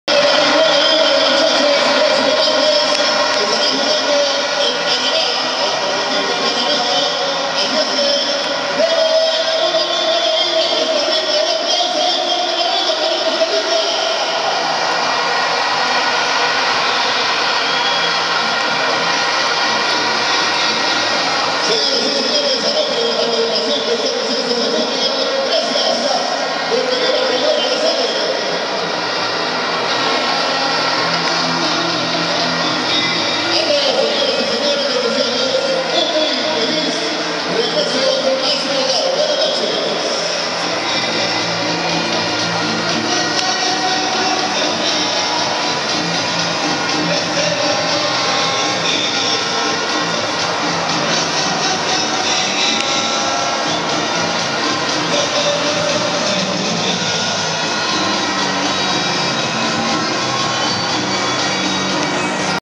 'Sele' festeja sufrido triunfo 1-0 sobre Haití al debutar en Premundial
Imágenes que recogen el festejo tico y el lamento haitiano, al concluir este viernes 13 de noviembre en el Estadio Nacional el primer duelo del grupo B, en la cuarta fase de la Concacaf, al Mundial Rusia 2018.